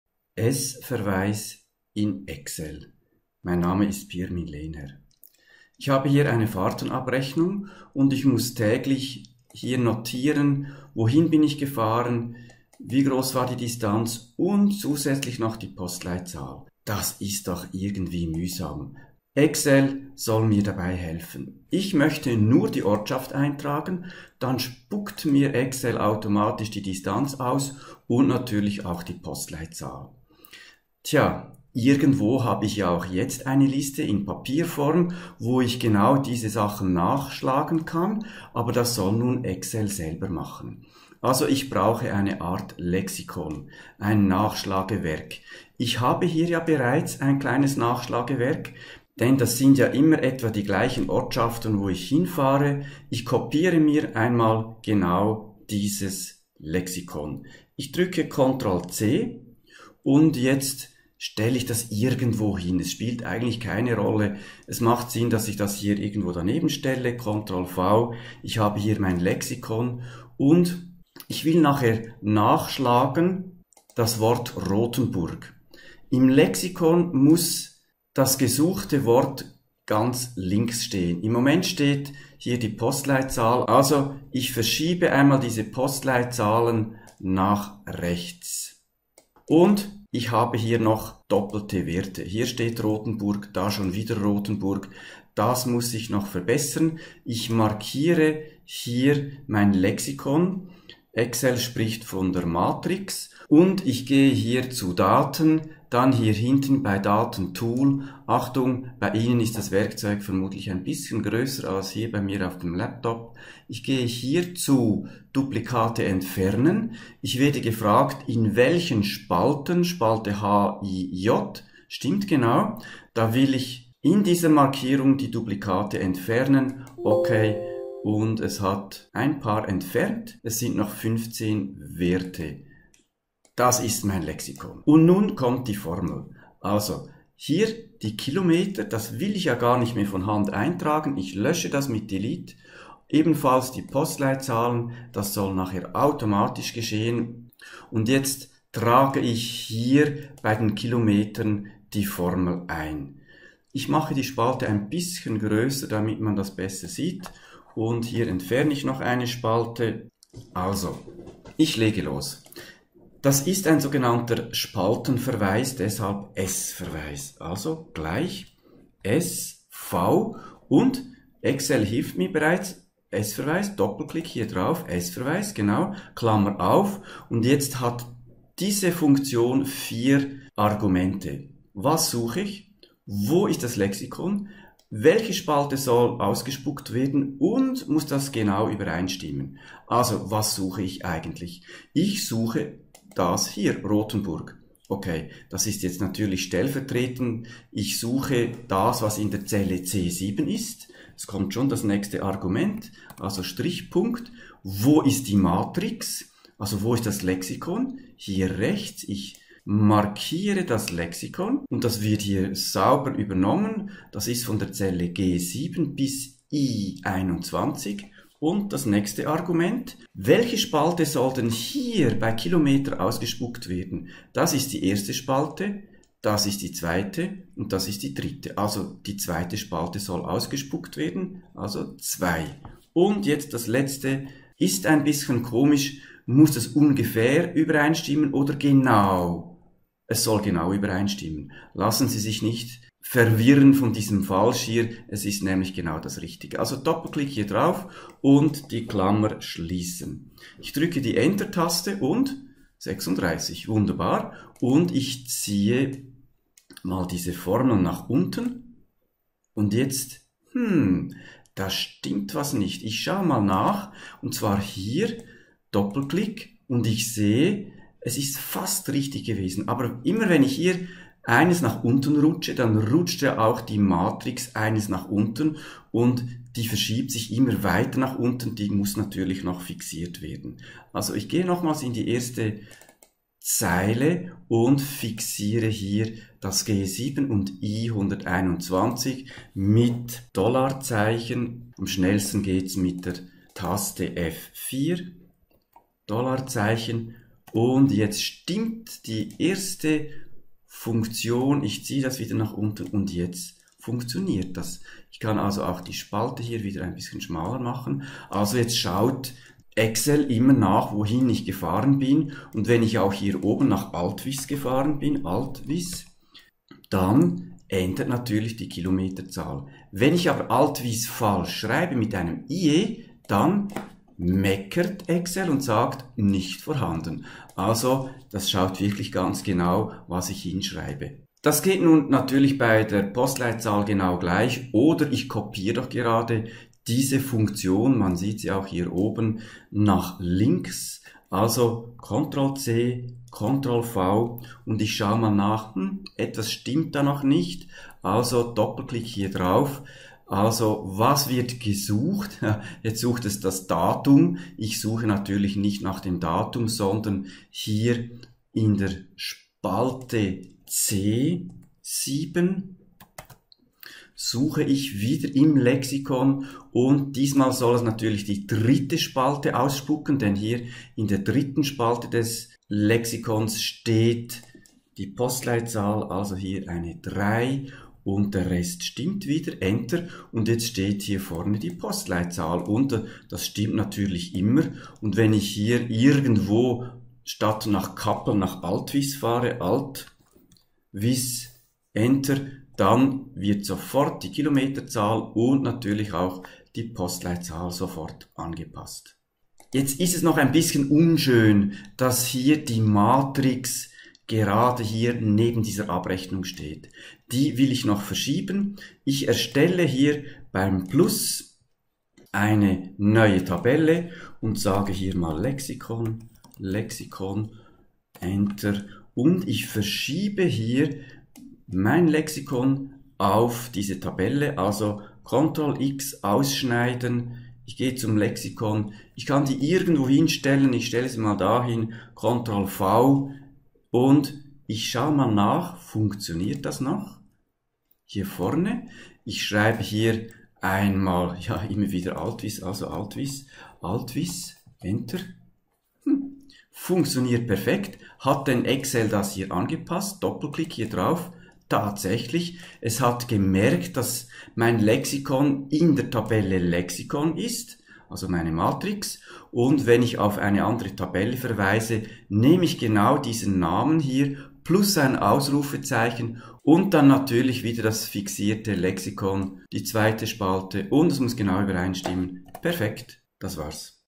Excel S-Verweis
Mit einem Spaltenverweis (S-Verweis) Daten auslesen und einfügen